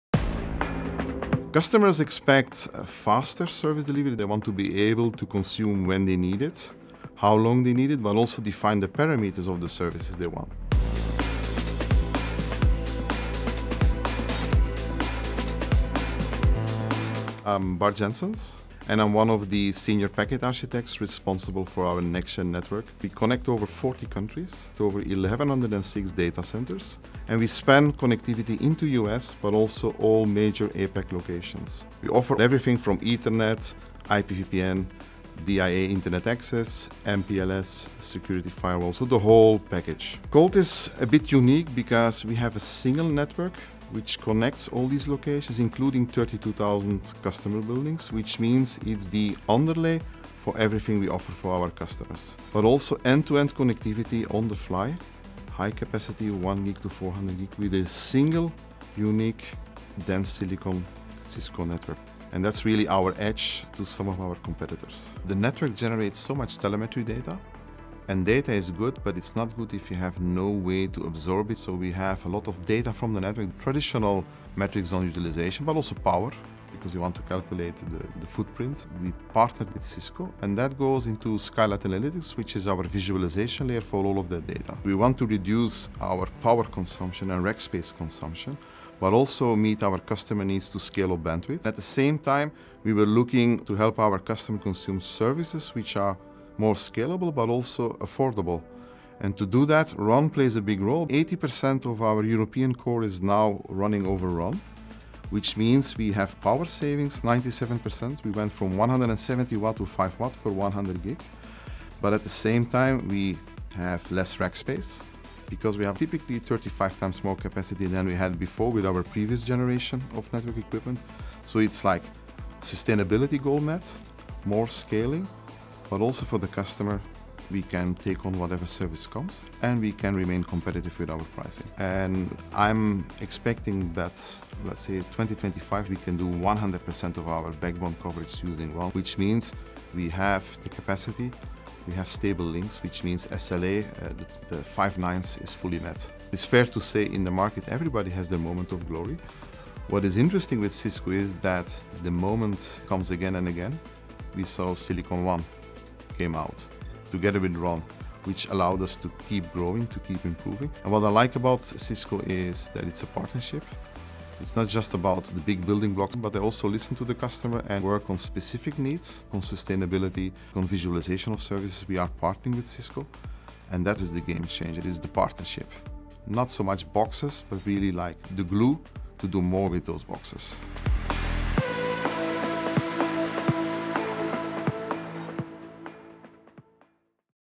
Efficient networks, empowered customers
Colt uses Cisco technology to help reduce energy consumption and provide scalable bandwidth to meet evolving enterprise customer needs.